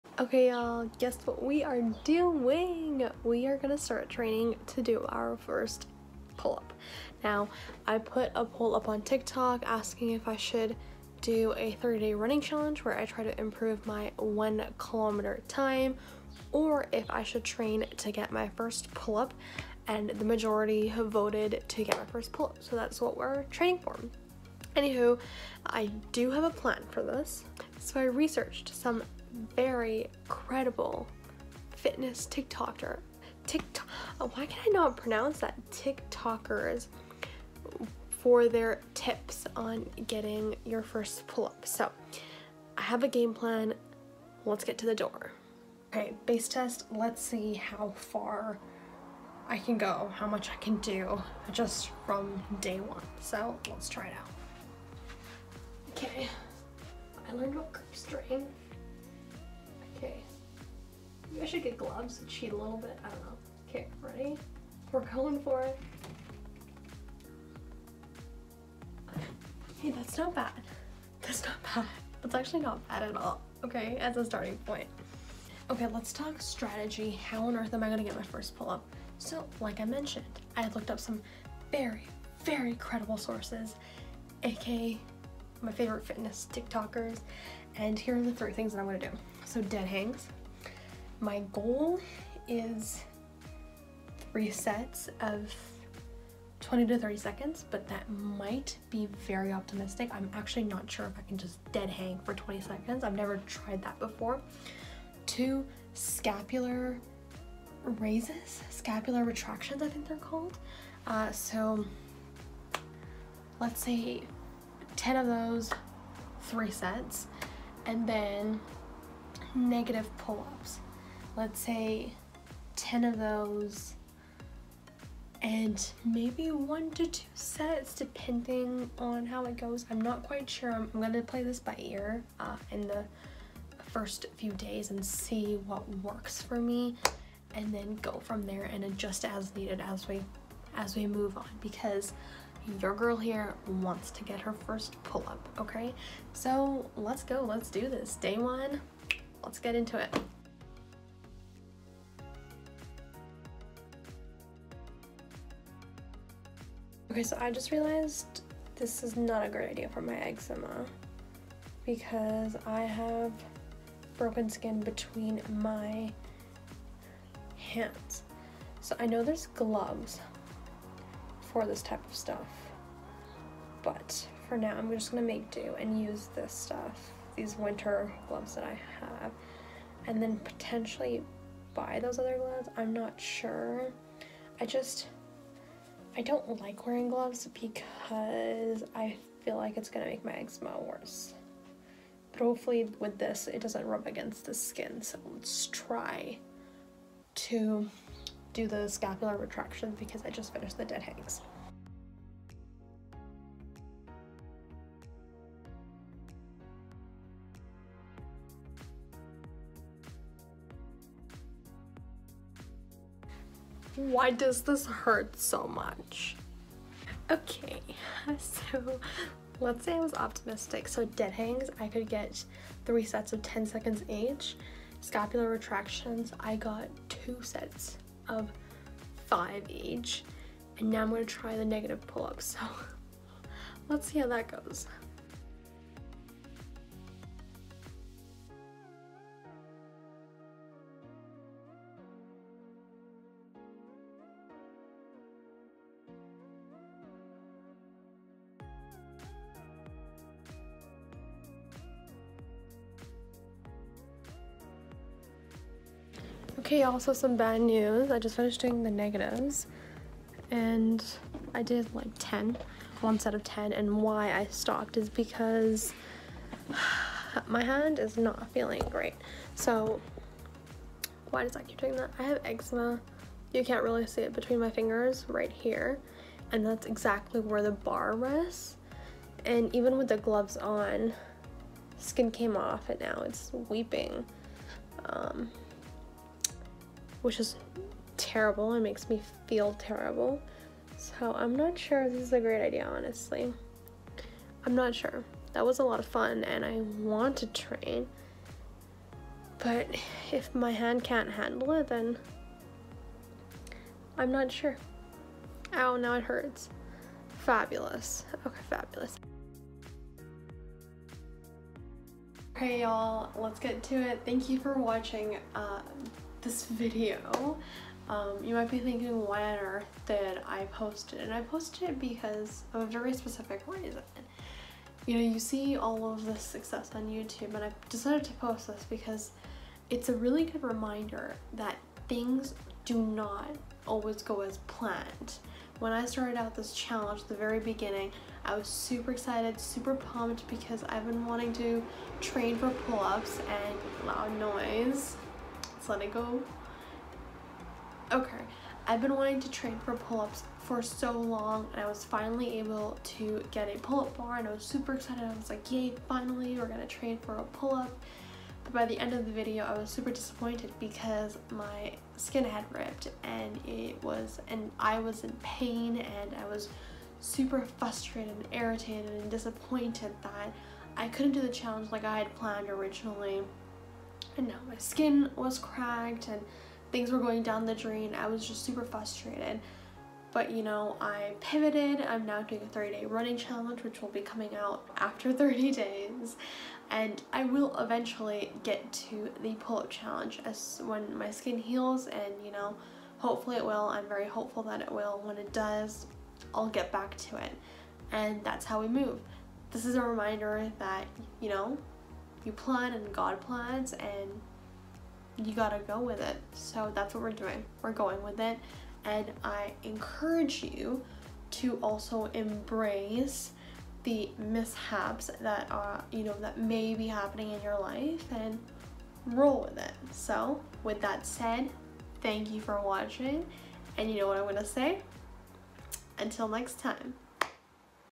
Pull up Training Challenge *FAIL*
Hi everyone, in today's video, I switch it up a bit. Recently, I started training to get my first pull up, but right from day 1 I realized that my hand eczema wouldn't be able to withstand the training. Even though I really wanted to get my first pull up, I had to pull back and pursue a different fitness goal (more coming on that later). It was frustrating and irritating, but I'm posting this so we all remember that things happen and life is taken in stride. We pivot as needed to grow and learn and have fun. It's okay and I'm hopeful in the future I'll be able to try this challenge again.